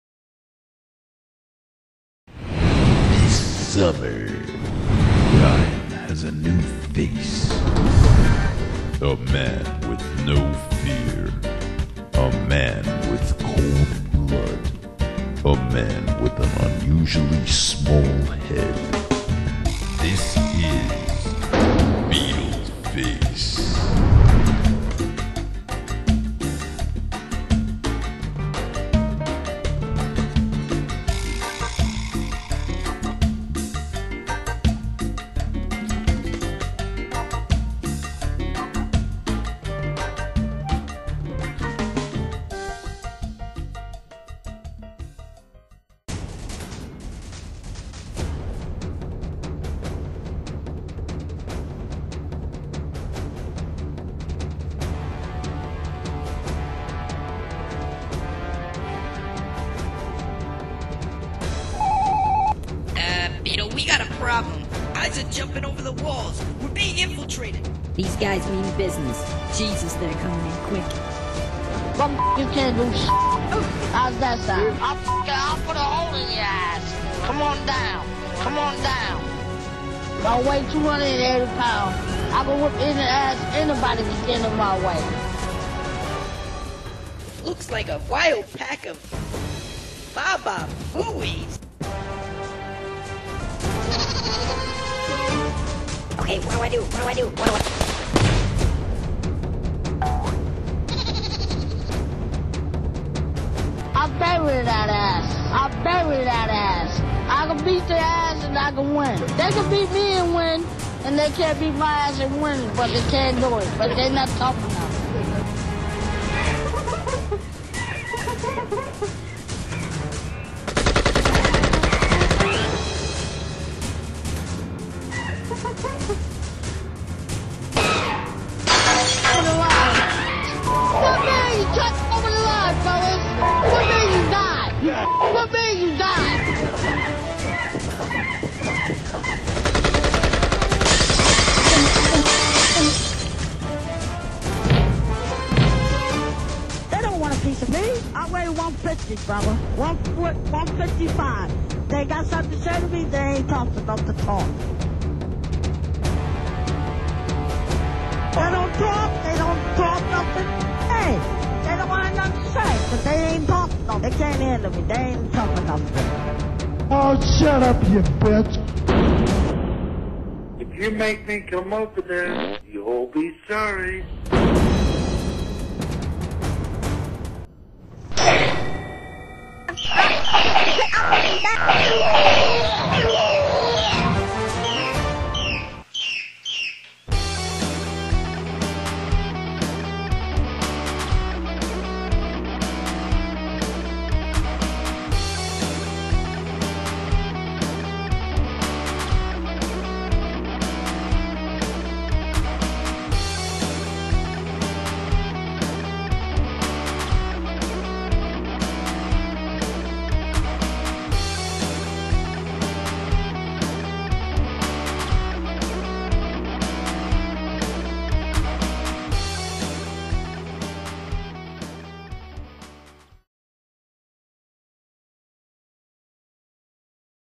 Beetleface (Beetlejuice from The Howard Stern Show)
Beetlejuice from the Howard Stern Show stars in "Beetleface". Pacino's got nothing on Beetlejuice. This was my entry into the Howard Stern Film Festival.